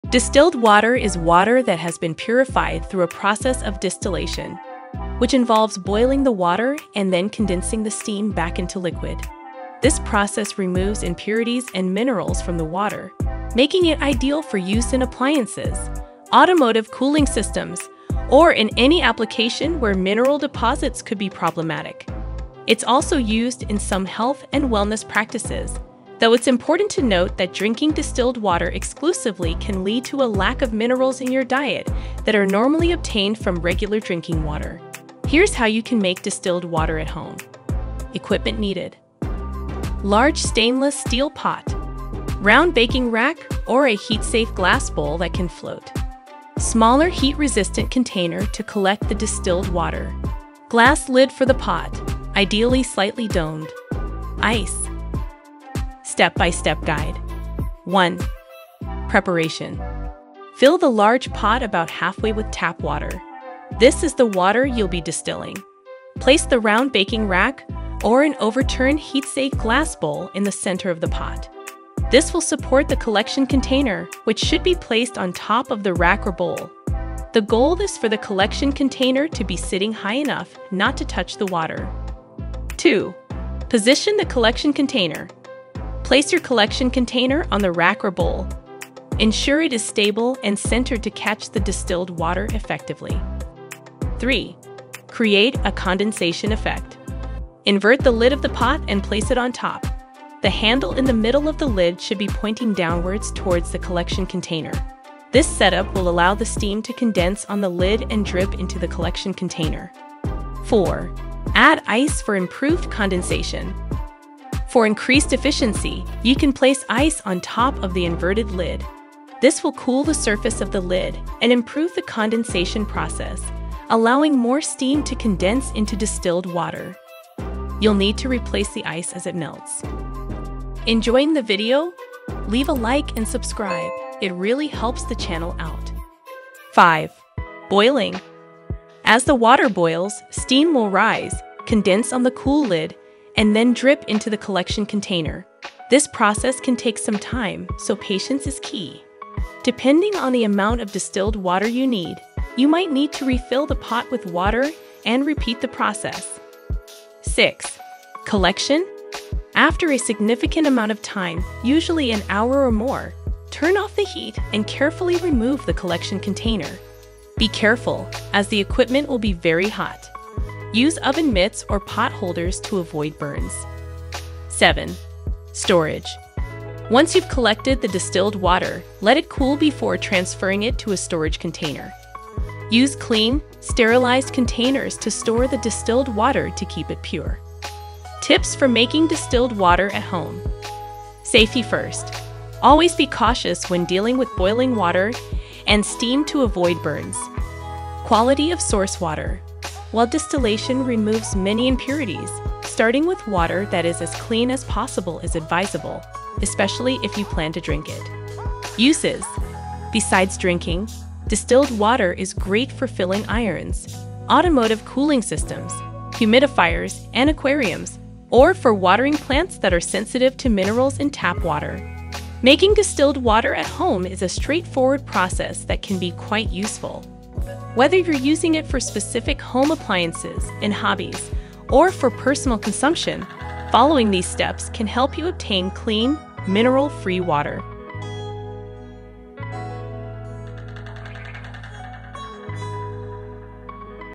How To Make Distilled Water at Home
How To Make Distilled Water at Home. Distilled water is water that has been purified through a process of distillation, which involves boiling the water and then condensing the steam back into liquid.

Our BEST Natural Hacks!

⏱️ Timestamps:
00:00 - Intro
00:38 - Equipment Needed
00:55 - Step-by-Step Guide
00:57 - Preparation
01:22 - Position the Collection Container
01:34 - Create a Condensation Effect
01:50 - Add Ice for Improved Condensation
02:16 - Boiling
02:37 - Collection
02:54 - Storage
03:08 - Tips for Making Distilled Water at Home
03:42 - Conclusion

📃 Video Summary:

Equipment Needed
- Large stainless steel pot
- Round baking rack or a heat-safe glass bowl that can float
- Smaller heat-resistant container (like a glass measuring cup) to collect the distilled water
- Glass lid for the pot, ideally slightly domed
- Ice

Step-by-Step Guide

1. Preparation
Fill the large pot about halfway with tap water. This is the water you'll be distilling. Place the round baking rack or an overturned heat-safe glass bowl in the center of the pot. This will support the collection container, which should be placed on top of the rack or bowl. The goal is for the collection container to be sitting high enough not to touch the water.

2. Position the Collection Container
Place your collection container (such as a glass measuring cup) on the rack or bowl. Ensure it is stable and centered to catch the distilled water effectively.

3. Create a Condensation Effect
Invert the lid of the pot and place it on top. The handle in the middle of the lid should be pointing downwards towards the collection container. This setup will allow the steam to condense on the lid and drip into the collection container.

4. Add Ice for Improved Condensation
For increased efficiency, you can place ice on top of the inverted lid. This will cool the surface of the lid and improve the condensation process, allowing more steam to condense into distilled water. You'll need to replace the ice as it melts.

5. Boiling
As the water boils, steam will rise, condense on the cool lid, and then drip into the collection container. This process can take some time, so patience is key. Depending on the amount of distilled water you need, you might need to refill the pot with water and repeat the process.

6. Collection
After a significant amount of time, usually an hour or more, turn off the heat and carefully remove the collection container. Be careful, as the equipment will be very hot. Use oven mitts or pot holders to avoid burns.

7. Storage
Once you've collected the distilled water, let it cool before transferring it to a storage container. Use clean, sterilized containers to store the distilled water to keep it pure.

Tips for Making Distilled Water at Home
• Safety First: Always be cautious when dealing with boiling water and steam to avoid burns.
• Quality of Source Water: While distillation removes many impurities, starting with water that is as clean as possible is advisable, especially if you plan to drink it.
• Uses: Besides drinking, distilled water is great for filling irons, automotive cooling systems, humidifiers, and aquariums, or for watering plants that are sensitive to minerals in tap water.

Making distilled water at home is a straightforward process that can be quite useful. Whether you're using it for specific home appliances, in hobbies, or for personal consumption, following these steps can help you obtain clean, mineral-free water.